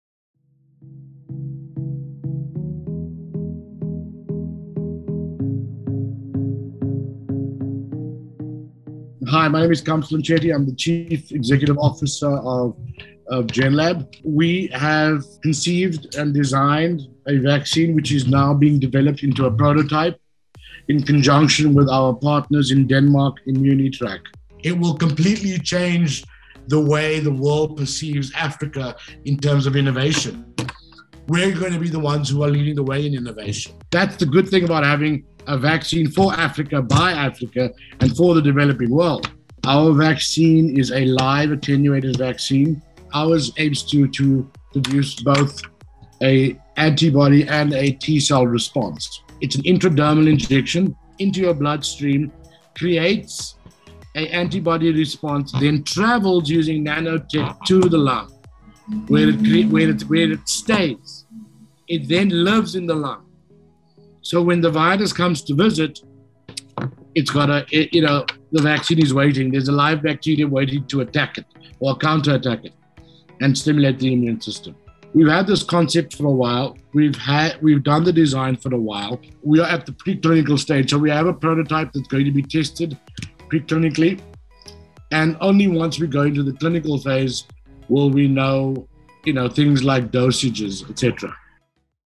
Shantivax - The first African-developed Covid-19 vaccine
A group of South African scientists have designed and developed the first Covid-19 vaccine of African origin, called Shantivax.
Port Elizabeth based immunotherapy company, Genlab, together with Danish biotechnology company, Immunitrack, have created a “next-generation vaccine” that will elicit both an antibody and T-cell response.